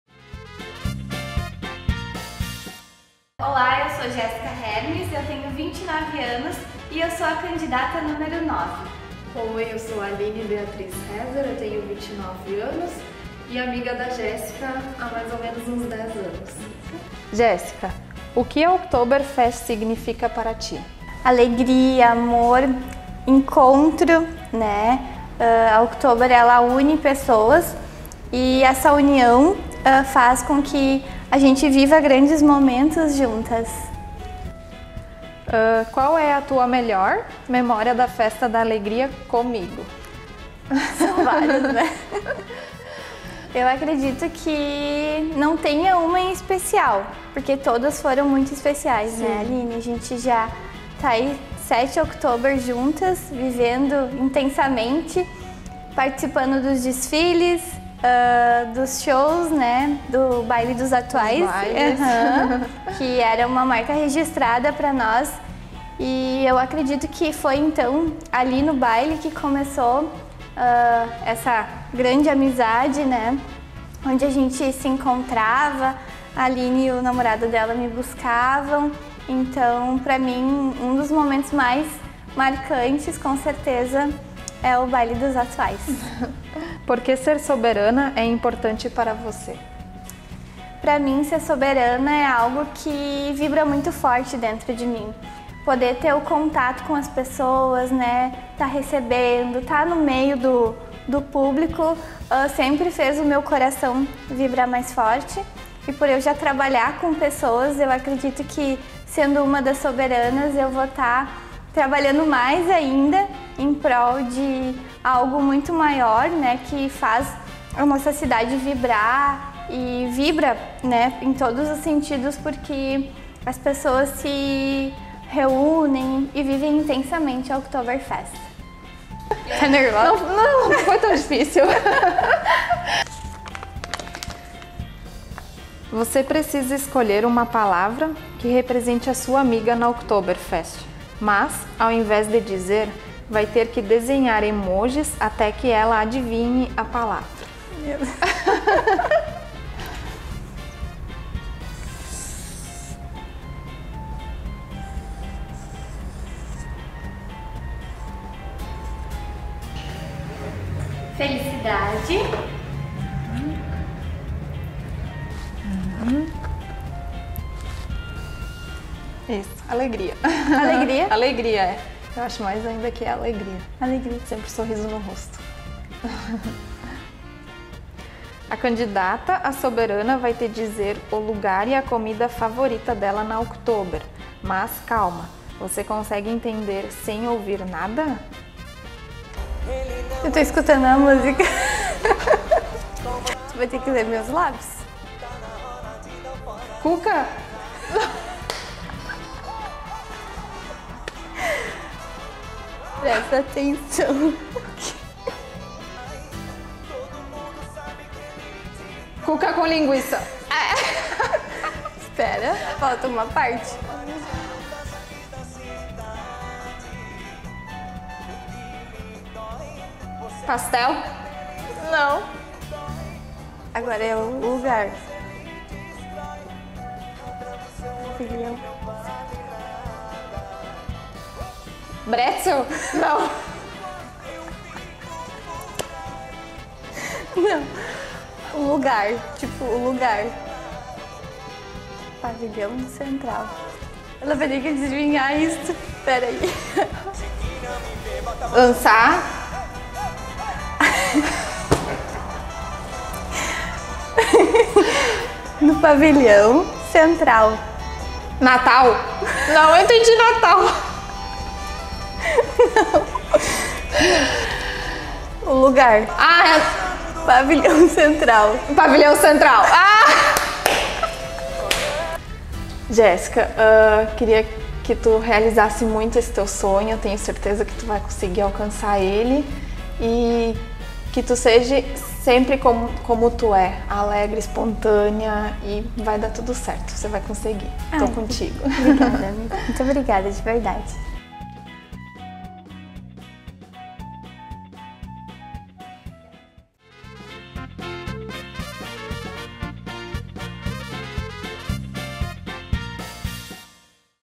Minha Amiga Soberana | Conheça a nona candidata à corte da 38ª Oktoberfest: Jéssica Hermes
📎Leia mais: